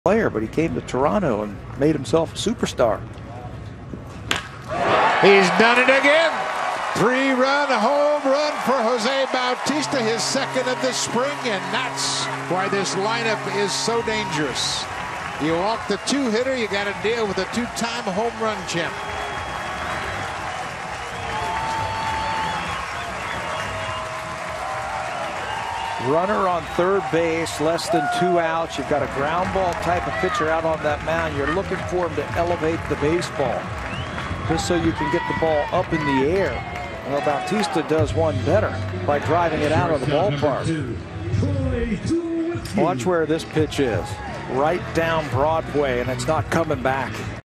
HOU@TOR: Bautista launches a long three-run shot
3/18/16: Jose Bautista sends a long blast over the left-field wall, giving the Blue Jays a 3-0 lead in the bottom of the 3rd